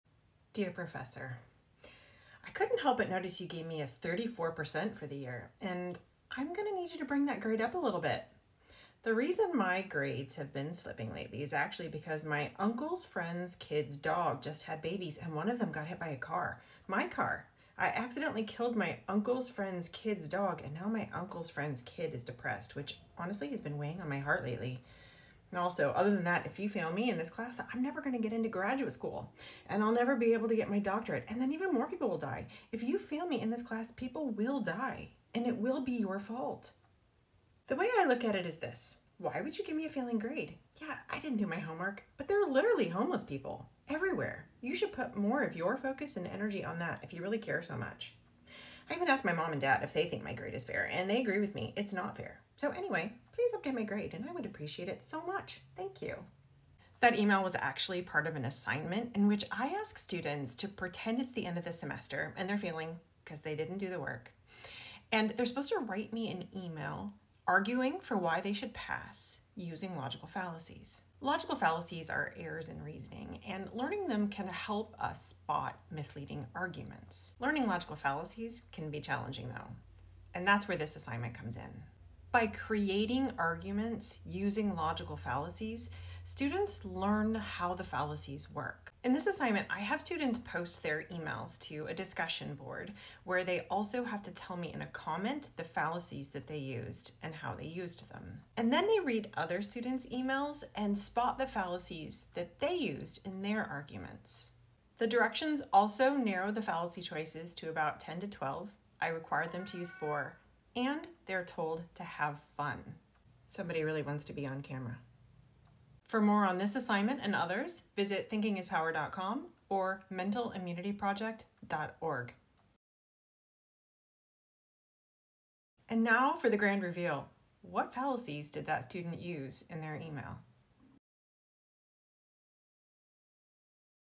Lesson Plan/Activity Intro: Logical Fallacies: Please Don't Fail Me
This lesson explores logical fallacies, those sneaky errors in reasoning that can warp our arguments and cloud our judgment. Through a fun and creative writing exercise, students use fallacies to create intentionally misleading (and humorous) arguments. They then analyze each others’ arguments to identify the fallacies and gain critical thinking skills to unmask deceptive claims in everyday life.